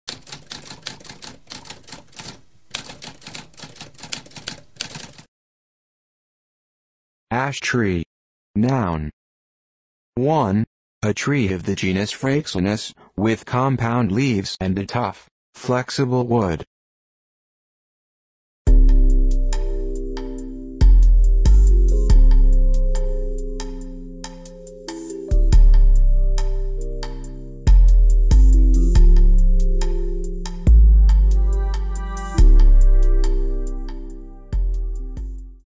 What does ash tree mean?
A spoken definition of ash tree.

Intro Sound:
Typewriter - Tamskp